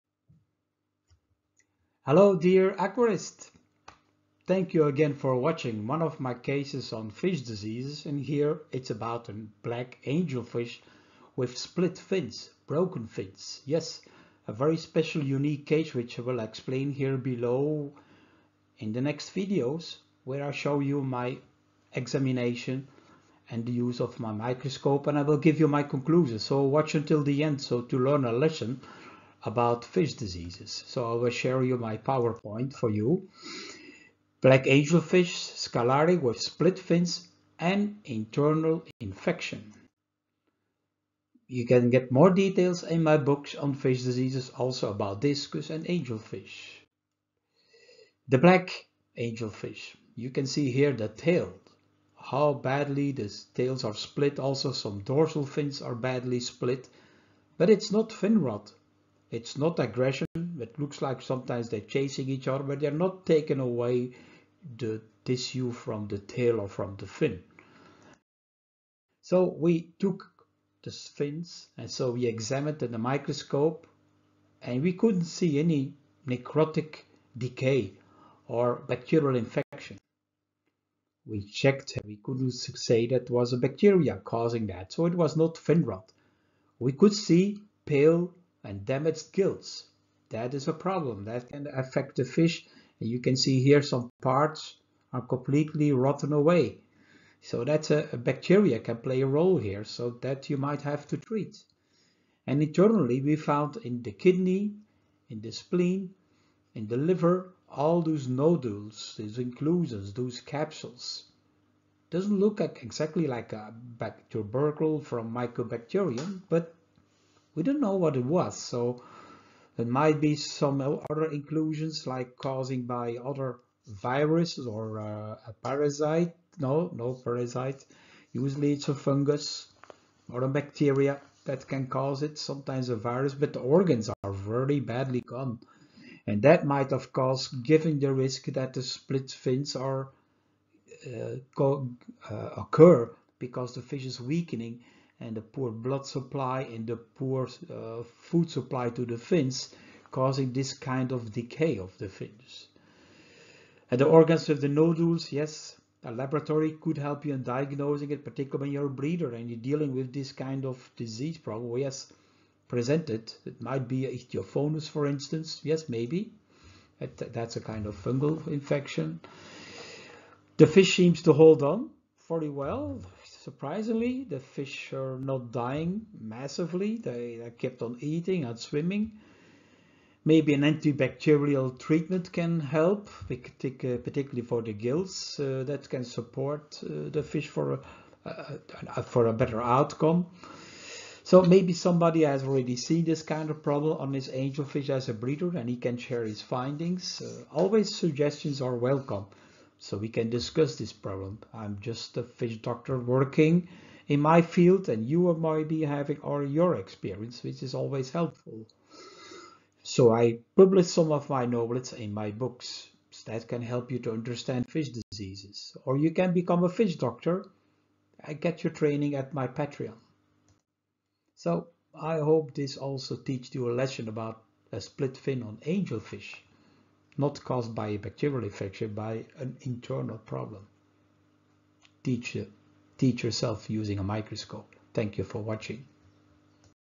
Fish disease case nr. 182 Angelfish Scalare with split fins, not finrot but internal infection
The black Angelfish,  Pterophyllum scalare, had for many weeks split fins (broken fins), especially the tails where badly split. We show our examination and diagnosis with the help of the microscope. The split fins where not finrot. We found an infection in the internal organs;

About split fins we made a special video= Split Fins

Learn more from my books (in different languages) on fish diseases (prevention, diagnosis and treatment) available